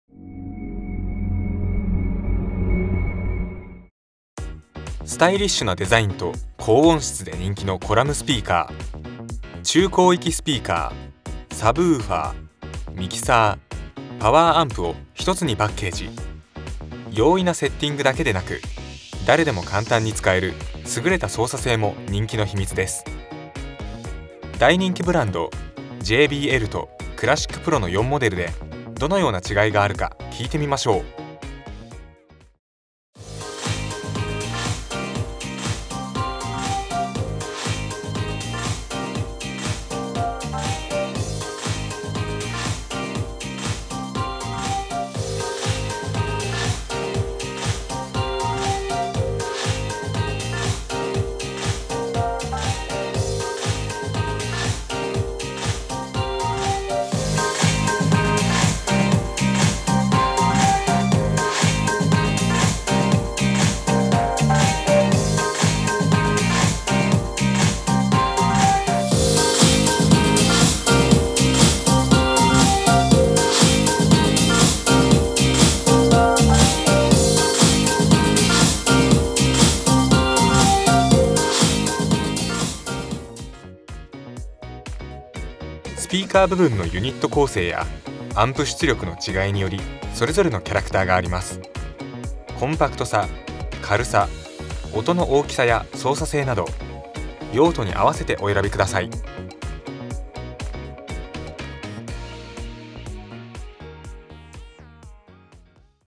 【JBL】人気コラムスピーカーサウンド比較【CLASSIC PRO】
CLASSIC PRO ( クラシックプロ ) / PA ONE　コラムスピーカー

CLASSIC PRO ( クラシックプロ ) / PA UNO　オールインワン・コラムスピーカー

JBL ( ジェービーエル ) / EON ONE PAセット イベント用

JBL ( ジェービーエル ) / EON ONE PRO PAセット イベント用

レコーダー：ZOOM/H6（MSマイクMSH-6を使用）
高さ：130㎝、スピーカーとの距離：200㎝に設置
同じ音源を使用し、各コラムスピーカーのCLIPが点灯しない程度に設定
一番レベルの大きなPA UNOにマイクの入力レベルを固定。

※EQは、4モデルとも全てフラットにしています。
※PA ONE、PA UNOのSUBWOOFER VOLUMEはMAXにしています。